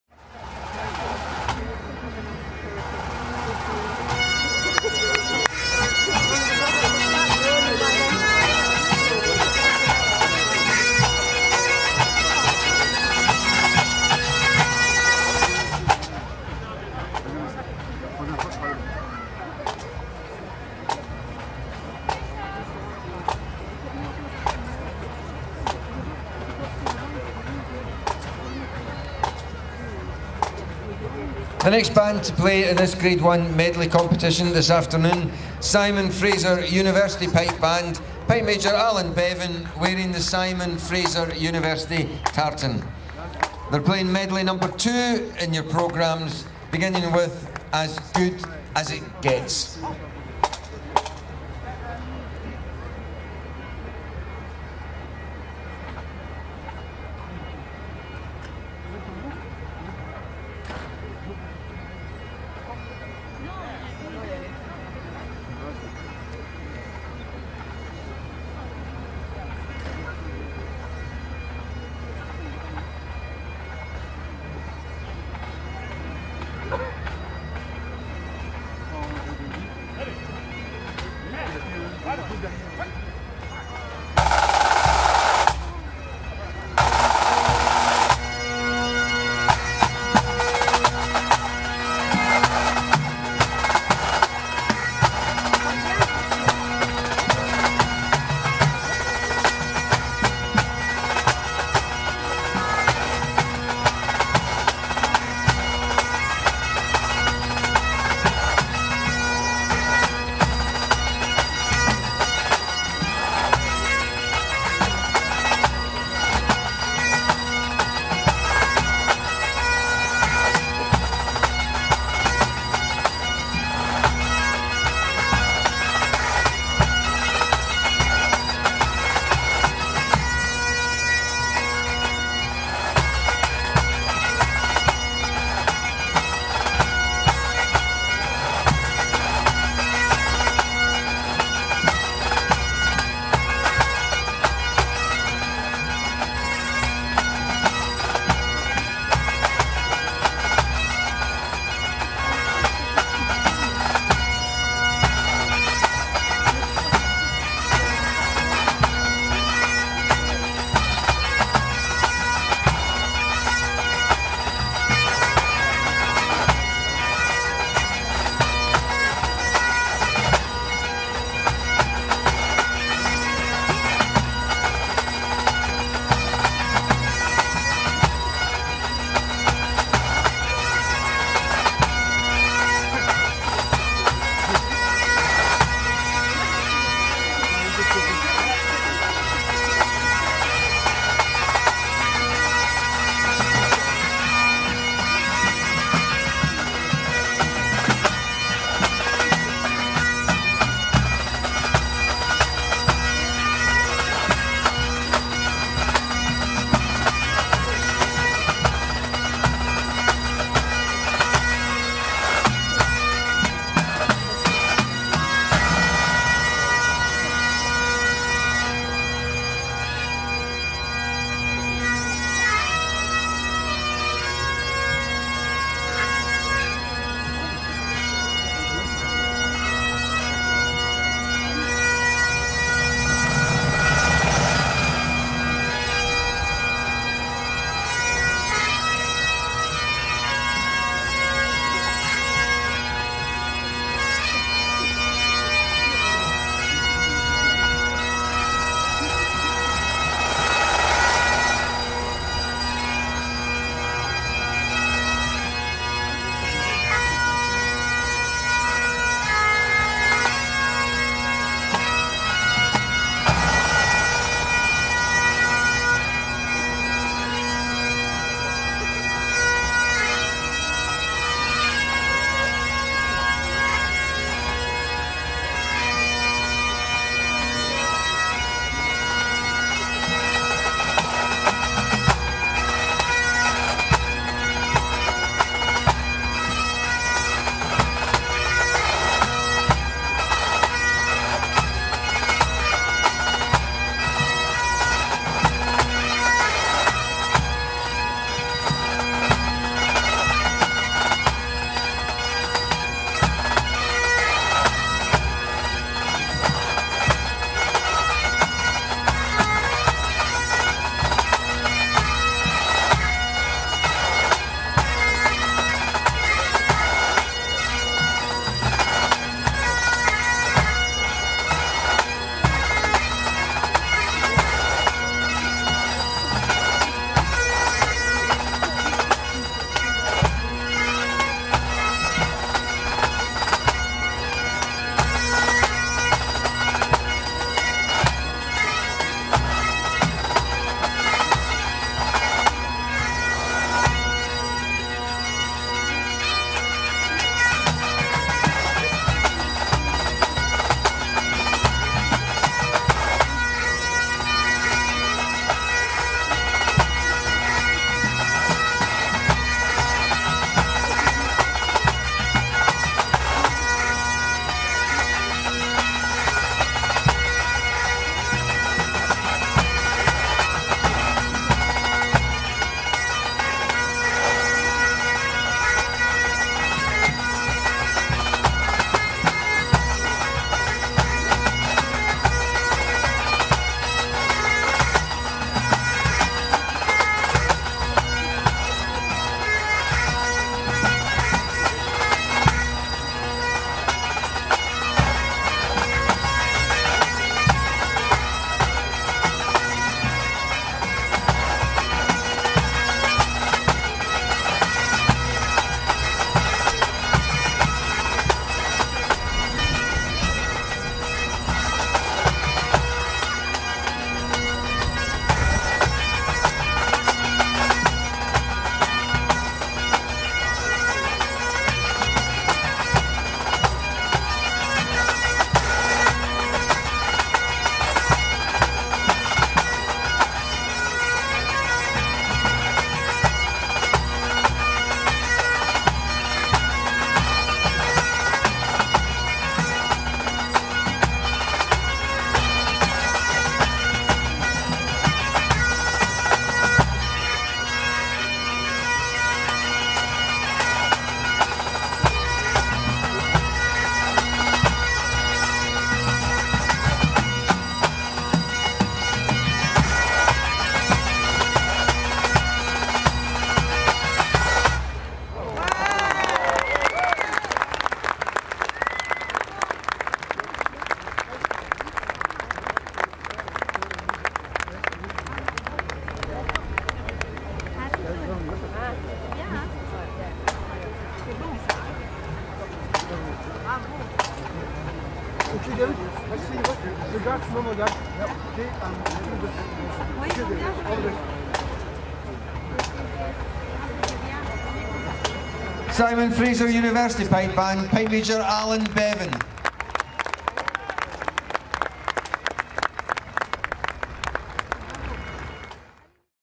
The WPB Championships 2019 - Simon Fraser University - Medley
It is now a full two years since the last World Pipe Band Championship at Glasgow Green thanks to this horrid pandemic. Let's hope 2022 "Worlds" goes ahead!